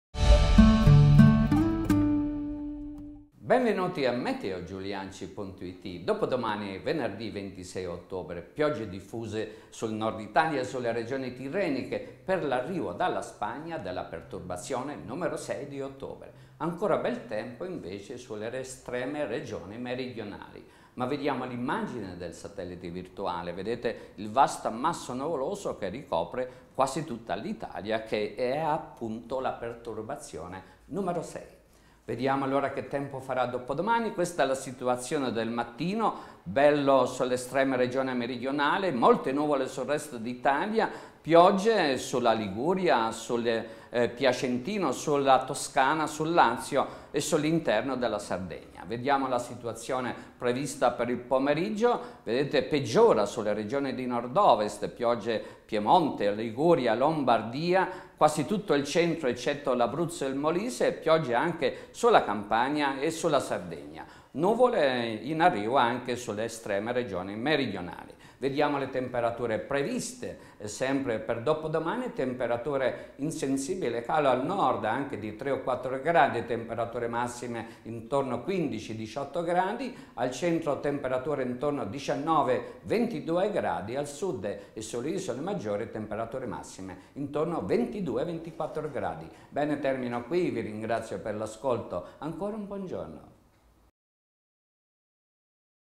Meteo di dopodomani, Venerdì 26 Ottobre 2012
Le previsioni del Colonnello Mario Giuliacci per dopodomani, Venerdì 26 Ottobre 2012